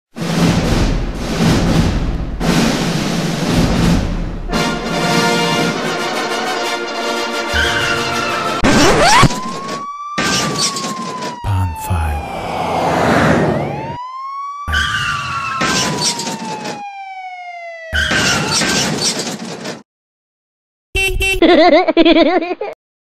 20th Century Fox Logo Has Been Taken Over By Cars!!!!
Uh oh, did the cars destruct those searchlights?!